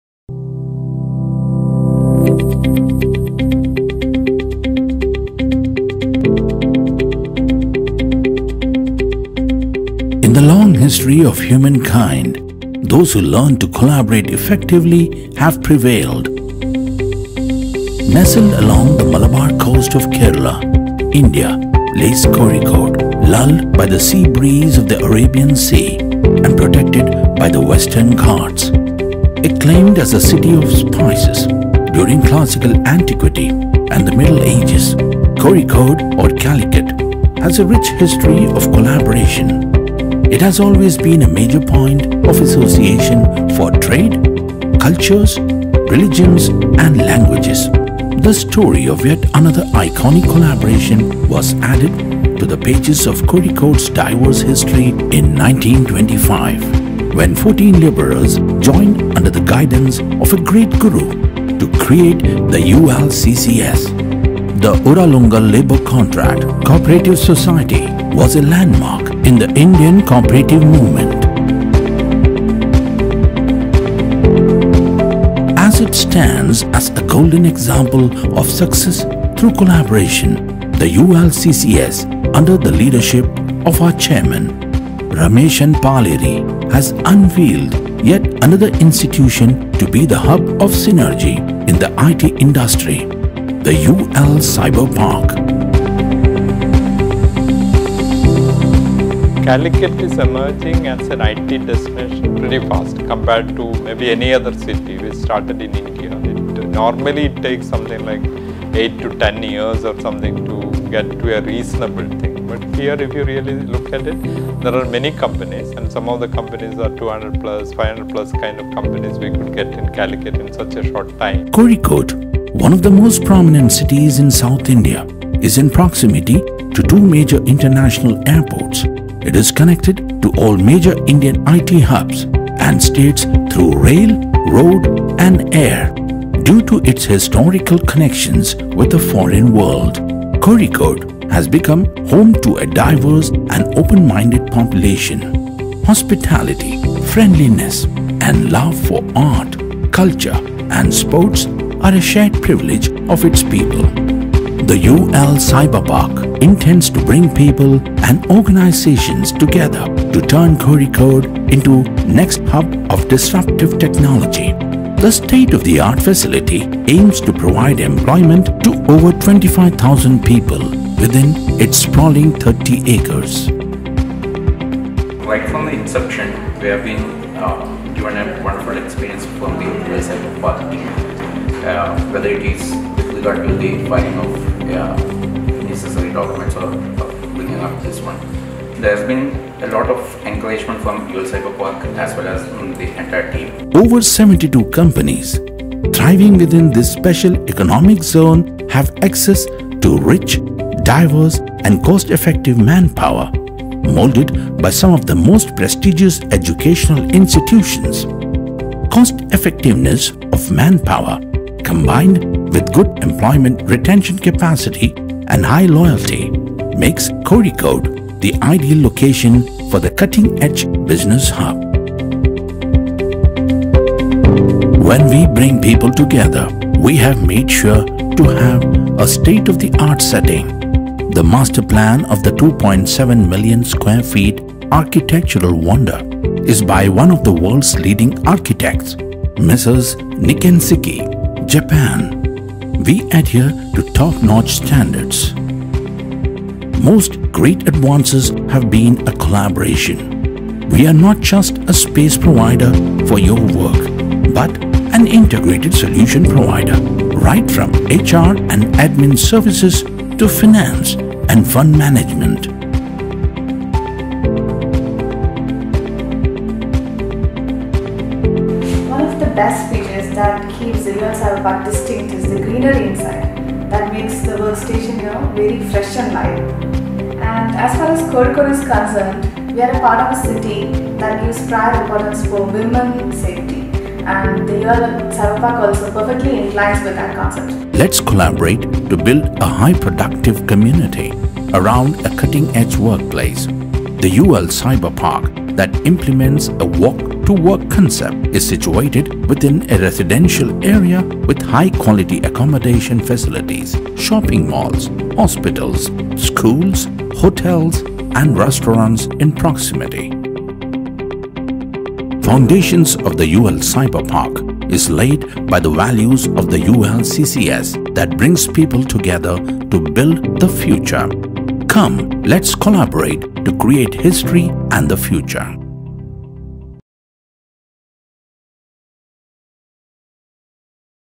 UL CyberPark: Let's Collaborate to Create History & The Future
Kozhikode or Calicut acclaimed as the ‘City of Spices’ during classical antiquity & the middle-ages has a rich history of collaboration. Here is yet another story of one such iconic collaboration that created a state-of-the-art IT infrastructure in North Kerala, the UL Cyberpark.

Narrated film- ULCyberpark. Created By team afterain.

DIRECTION: NijeeshVijay
SCRIPT:ArunKishore
DOP:Himal Mohan, Nijeesh, Shiju Thiruvalloor
EDIT:VinilJith,
GRAPHICS: Arun C K, Rahul Raghav

CONTENT SUPPORT: SitharaSunil
ASST. DIRECTION: Prajeesh Kolora
PROJECT SUPPORT: RemyaNijeesh, Shybu AP, Dinesh
NARRATION:SANTOSH ABRAHAM
PRODUCTION CONTROLLER : Dineesh Kavil
PRODUCTION HOUSE: afterain vfx box